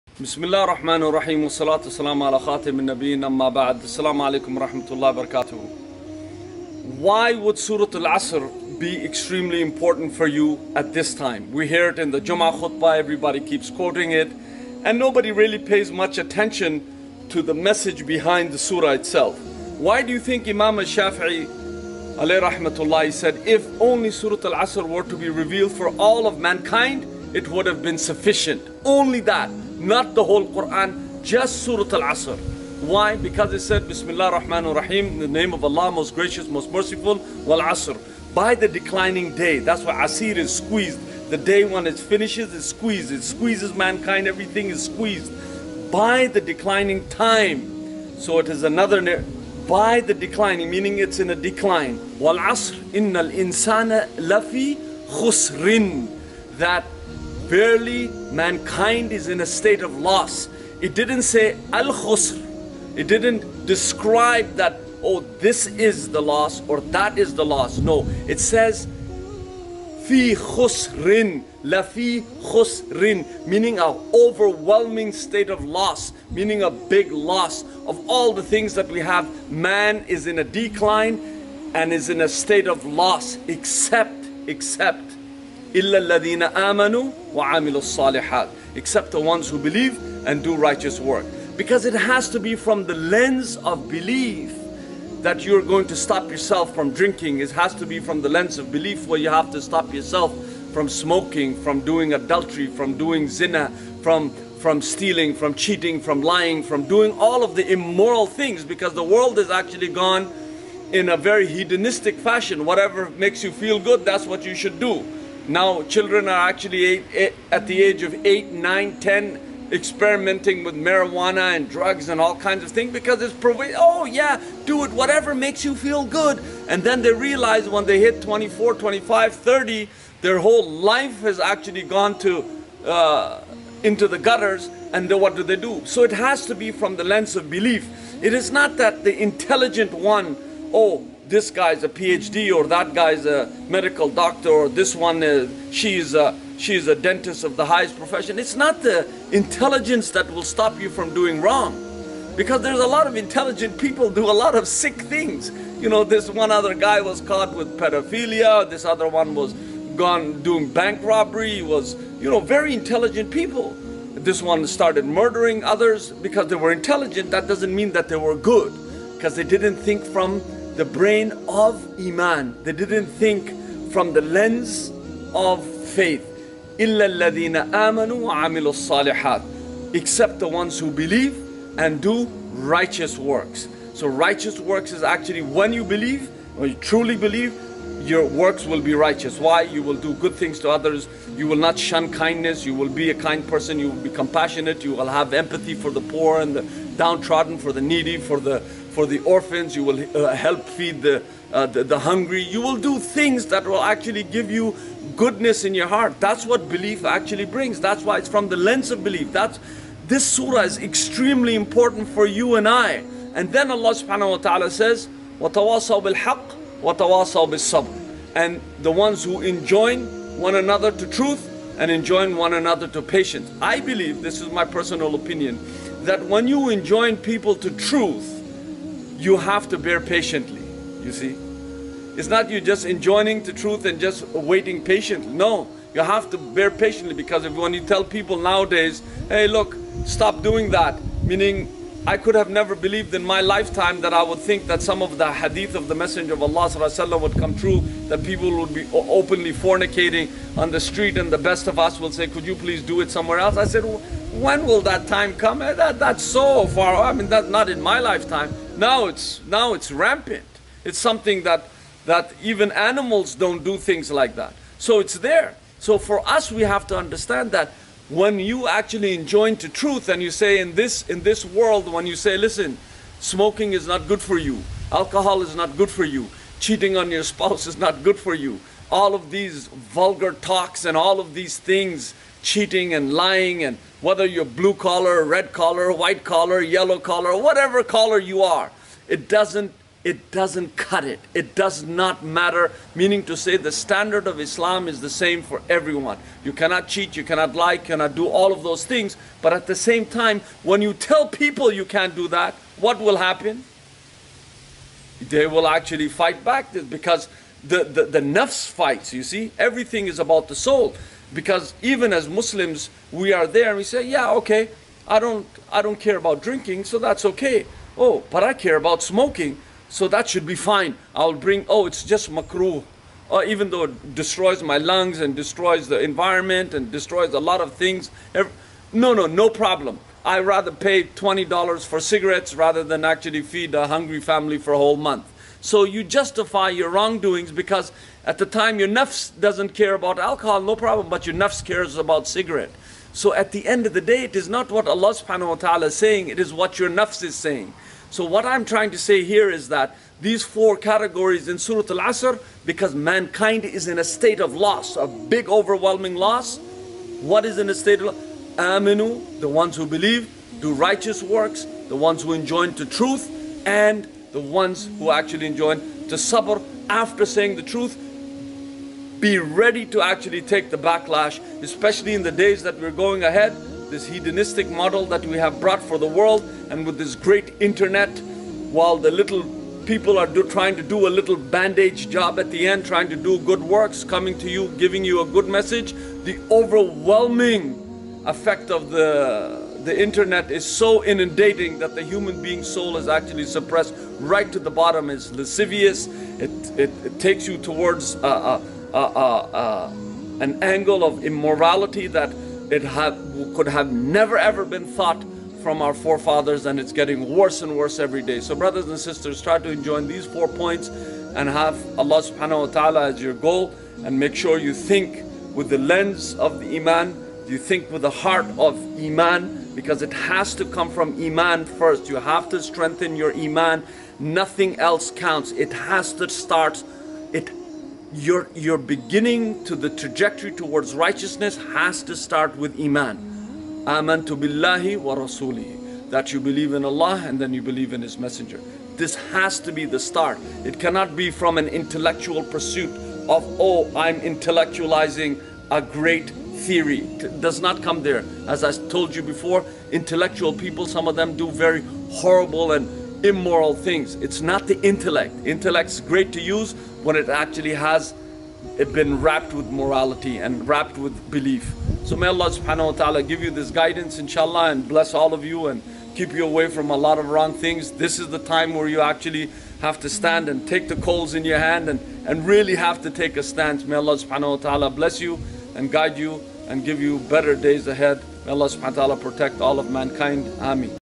SURAH AL-ASR IS ENOUGH FOR HUMANKIND'S GUIDANCE!
Surah Al Asr is enough for humankind's guidance. This short surah is packed with instructions sufficient for all of us.